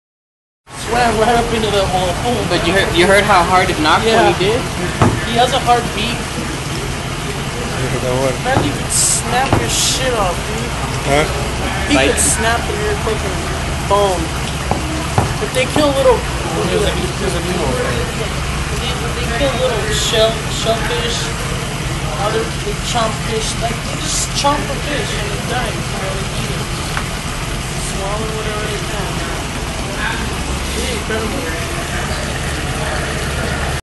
Loggerhead Sea Turtle
Loggerhead Sea Turtle @ Sea Turtle Rescue center In South Padre Island that was hit by a boat and lost its leg.